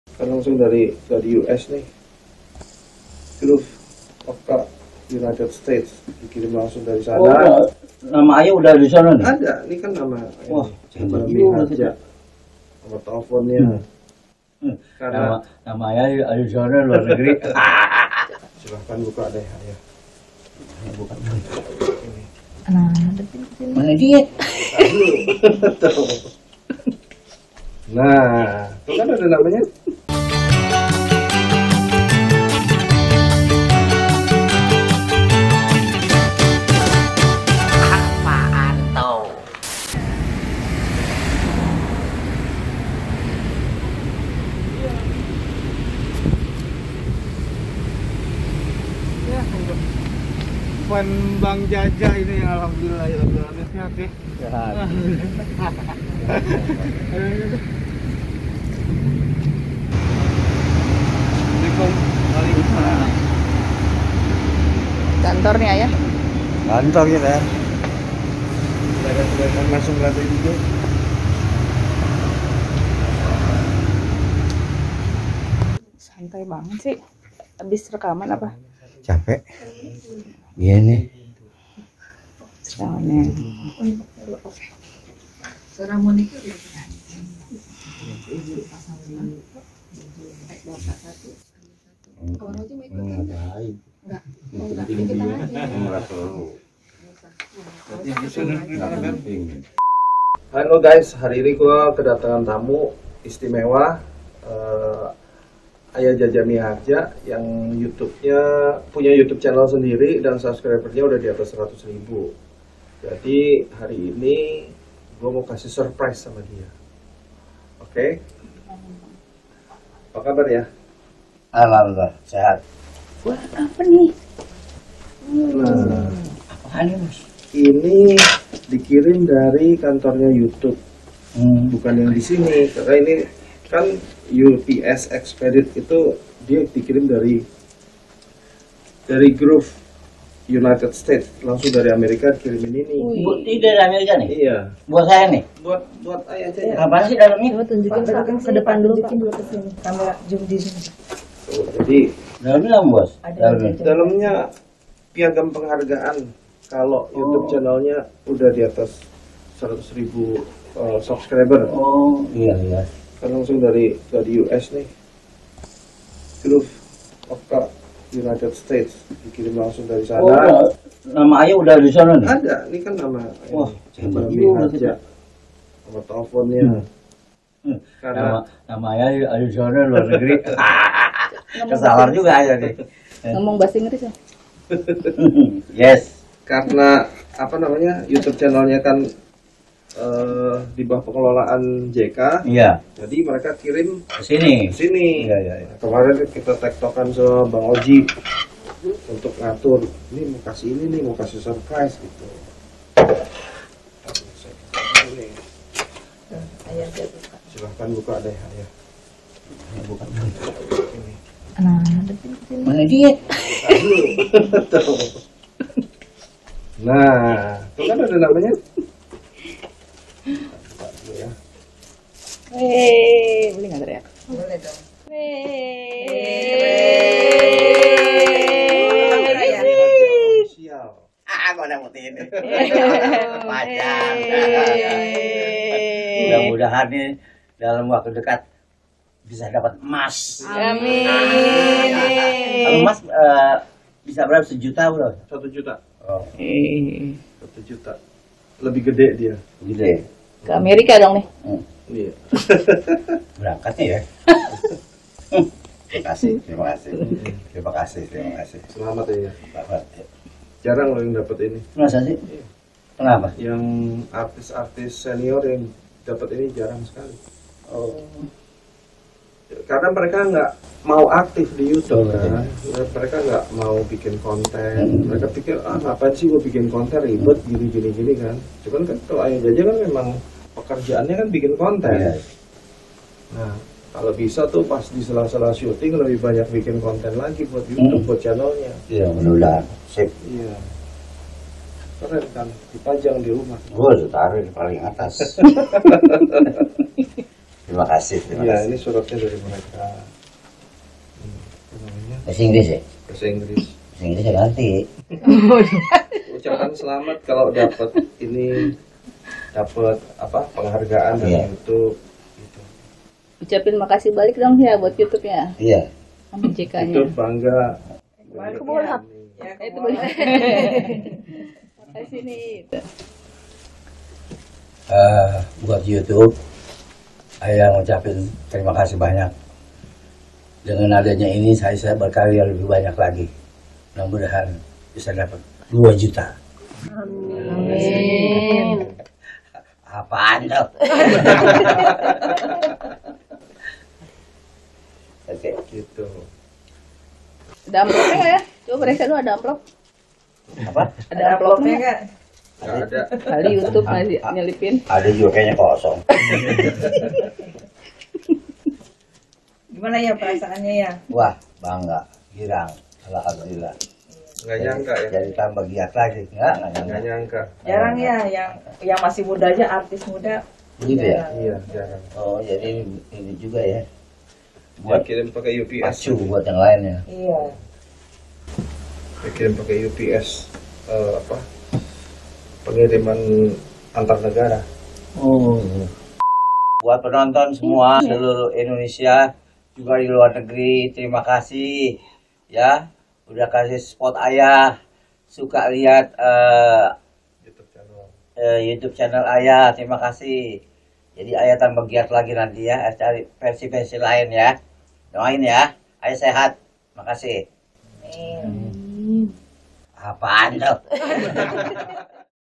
JAJA MIHARJA | NAMA AYAH JAJA SAMPE AMERIKA | SILVER BUTTON
Terima kasih untuk semua subscriber setia ayah Jaja Mihardja Official berkat support kalian semua ayah dapet silver button. Mohon doa ayah sehat selalu serta panjang umur dan makin semangat bikin konten untuk menghibur dan mengedukasi para penonton semua. Semoga kedepan bisa dapet gold button secepatnya. Aamiin.

Prod.Exc: Vita Miharja
Producer: Nurfaiza Miharja
Creative : Radi / Ferdi
Editor     : Yogi
Camera  : Ozzy / Vita
Costume: Khalisa / Radiv
Director : Ozzy Miharja
Prod       : JM marcomm